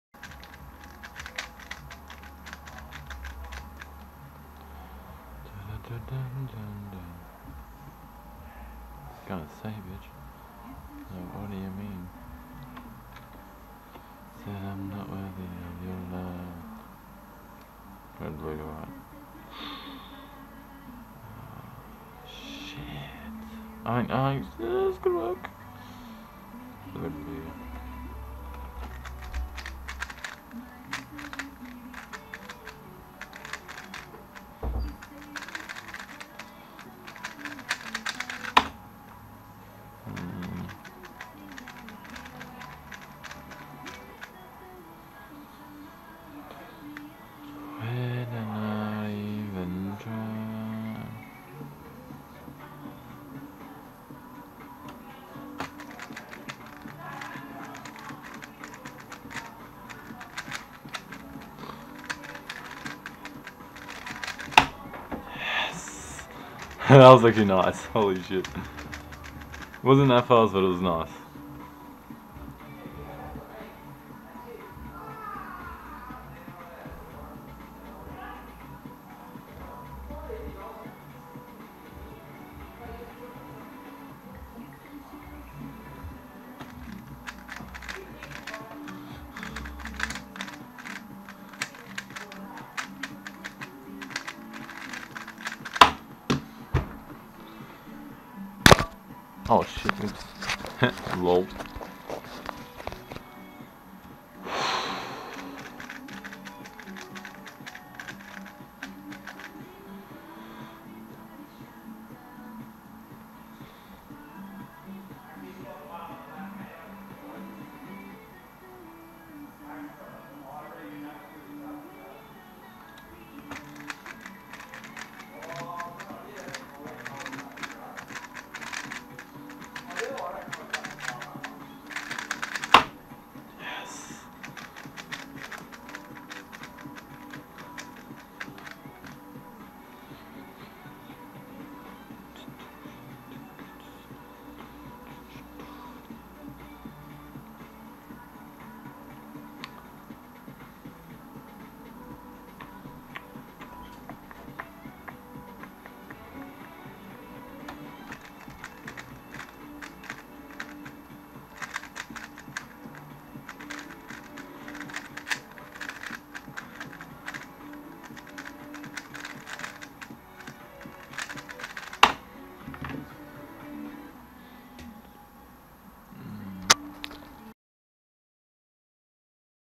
9.40sec 3x3 average of 5
Just a nice average I felt like uploading. Only because I feel bad for not uploading in so long. Once again I will not let this happen again.